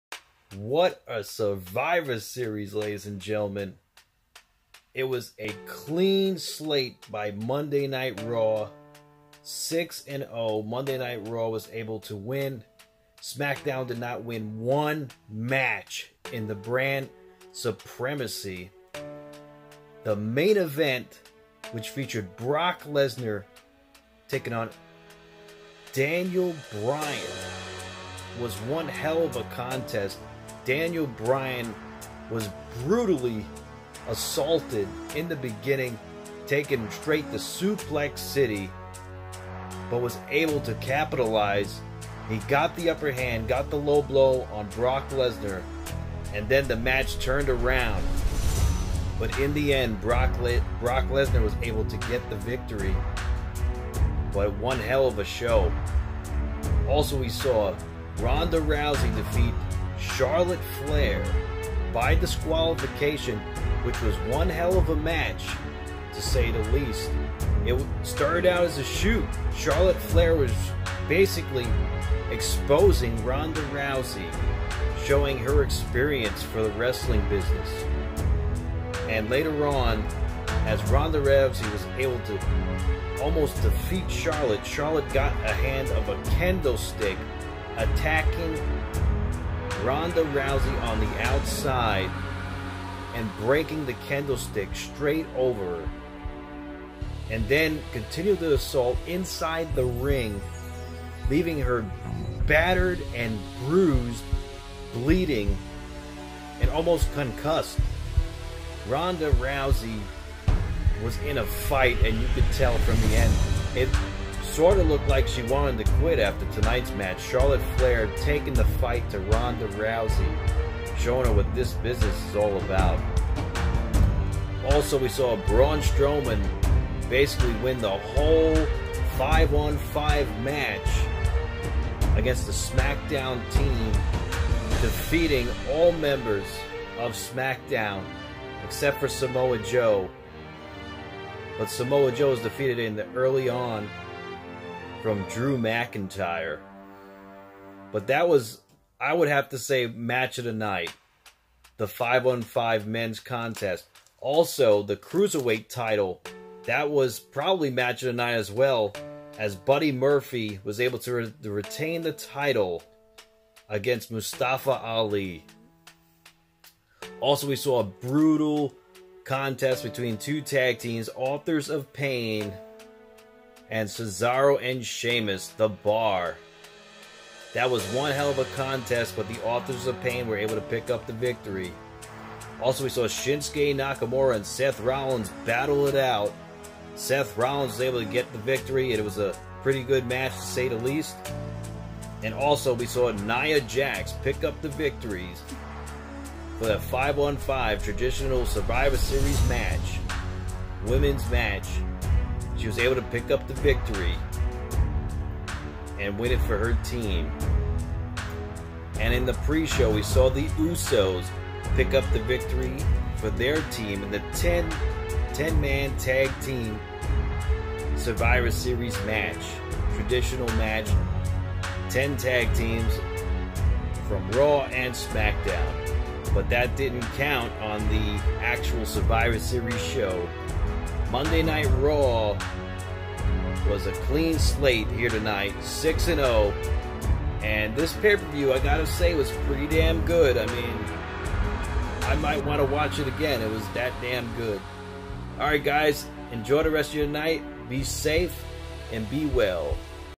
WWE SURVIVOR SERIES REVIEW 11/18/2018
WWE SURVIVOR SERIES RECAP Highlights Discussed & Reviewed 2018

Brock Lesnar defeated Daniel Bryan in the Main Event. Daniel Bryan put up one hell of a fight but in the end Lesnar was too strong.

Ronda Rousey won by disqualification against Charlotte Flair. Charlotte brutally attacked and assaulted Ronda Rousey with a Kendo Stick and busted her open. Ronda was bruised and battered. Charlotte snapped on Ronda Rousey and made her bleed and left scars and bruises all over her body.

Team Raw Defeated Team Smackdown 5 on 5 on both the Men's and Women's sides. Braun Strowman Picking up the victories and Nia Jaxxx.

Match of the Night Buddy Murphy retained the crusierweight title against Mustafa Ali.

The Authors of Pain Defeated The Bar Sheamus and Cesaro.

Seth Rollins won against Shinske Nakamura.

Survivor Series Preshow The Usos won for their 10 man tag team against Raw.

Raw won the Supremacy 3rd year in a row. 6-0. No Smackdown competitor won tonight.